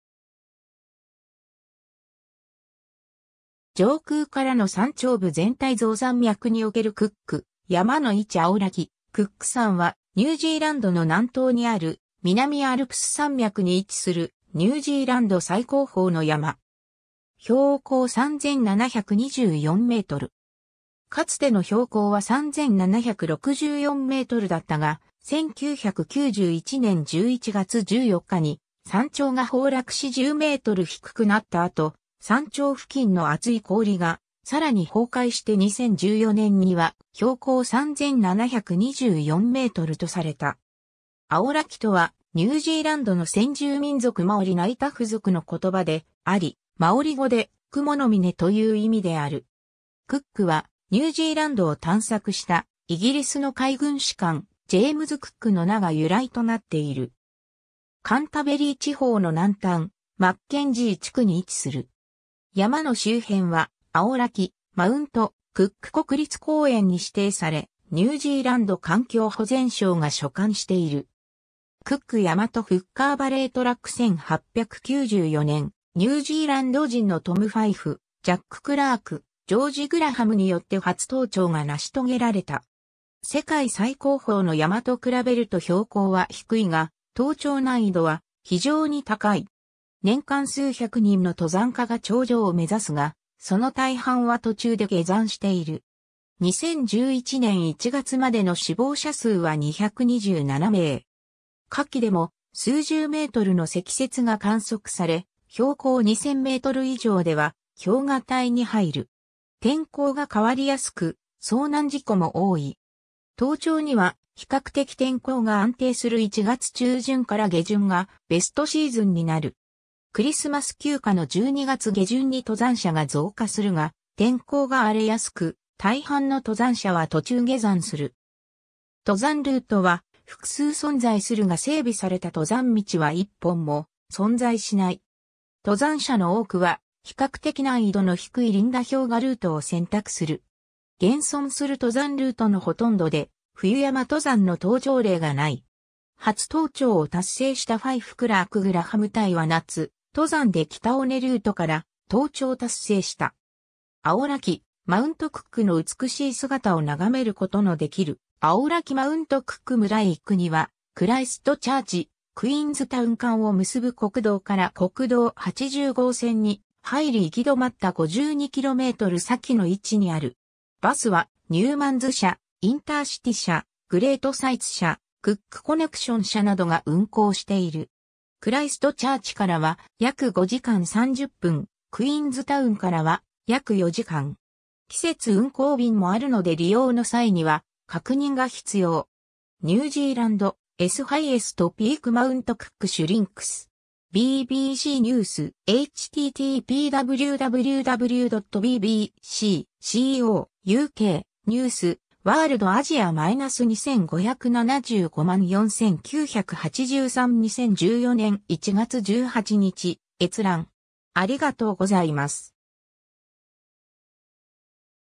クック山
南島_(ニュージーランド)
ジェームズ・クック
上空からの山頂部 全体像 山脈におけるクック山の位置 アオラキ / クック山（クックさん、マオリ語: Aoraki、英: Mount Cook）は、ニュージーランドの南島にある南アルプス山脈に位置するニュージーランド最高峰の山。
標高3,724メートル。
かつての標高は3,764メートルだったが、1991年11月14日に山頂が崩落し10メートル低くなった後、山頂付近の厚い氷がさらに崩壊して2014年には標高3,724メートルとされた。
 「アオラキ（Aoraki）」とは、ニュージーランドの先住民族マオリ・ナイ・タフ族（Kāi Tahu）の言葉であり、マオリ語で「雲の峰」という意味である。
「クック」はニュージーランドを探索したイギリスの海軍士官ジェームズ・クックの名が由来となっている。
 カンタベリー地方の南端マッケンジー地区に位置する。
山の周辺はアオラキ/マウント・クック国立公園に指定され、ニュージーランド環境保全省が所管している。
 クック山とHooker Valley Track 1894年、ニュージーランド人のトム・ファイフ、ジャック・クラーク、ジョージ・グラハムによって初登頂が成し遂げられた。
 世界最高峰の山と比べると標高は低いが、登頂難易度は非常に高い。
年間数百人の登山家が頂上を目指すが、その大半は途中で下山している。
2011年1月までの死亡者数は227名。
 夏季（12～2月）でも数十メートルの積雪が観測され、標高2000メートル以上では氷河帯に入る。
天候が変わりやすく遭難事故も多い。
登頂には比較的天候が安定する1月中旬から下旬がベストシーズンになる。
クリスマス休暇の12月下旬に登山者が増加するが天候が荒れやすく、大半の登山者は途中下山する。
 登山ルートは複数存在するが整備された登山道は1本も存在しない。
登山者の多くは比較的難易度の低いリンダ氷河ルートを選択する。
現存する登山ルートのほとんどで冬山登山の登頂例がない。
初登頂を達成したファイフ・クラーク・グラハム隊は夏登山で北尾根ルートから登頂達成した。
 アオラキ/マウントクックの美しい姿を眺めることのできるアオラキマウントクック村へ行くには、クライストチャーチ、クイーンズタウン間を結ぶ国道から国道80号線に入り行き止まった52km先の位置にある。
 バスは、ニューマンズ社、インターシティ社、グレートサイツ社、クック・コネクション社などが運行している（1日1便程度の共同運行）。
 クライストチャーチからは約5時間30分、クイーンズタウンからは約4時間。
季節運行便もあるので利用の際には確認が必要。
^ “New Zealand's highest peak Mount Cook shrinks” (英語). BBC News (BBC). 2014年1月18日閲覧。